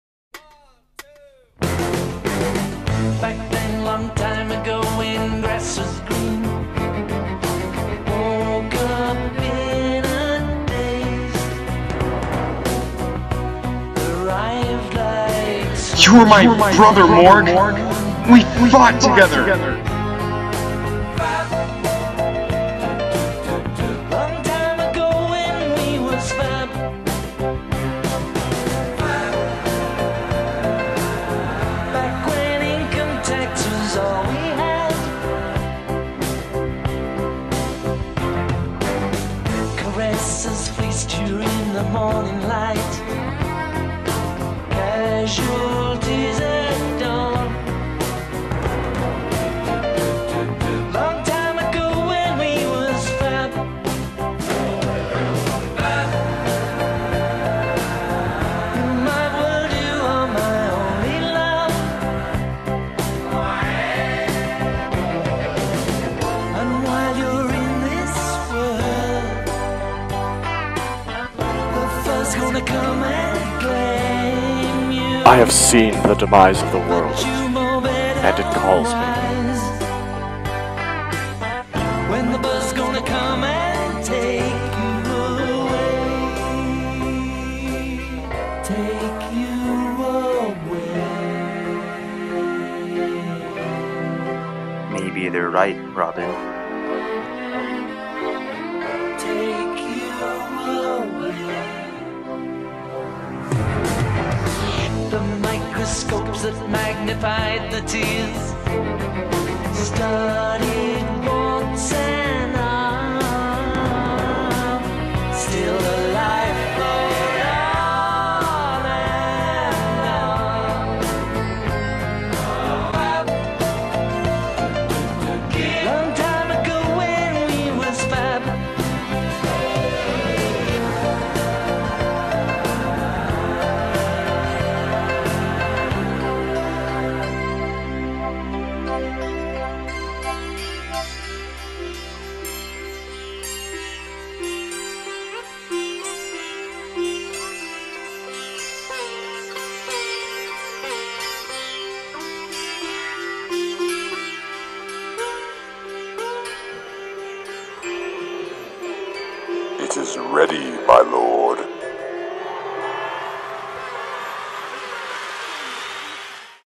The Robin Tyler SMP Official Trailer
The Robin Tyler SMP will be coming to Youtube.com near you.
Featuring:
Robin Tyler
Son of Morg
The Sapphire Knight
Co-Dizzle
and more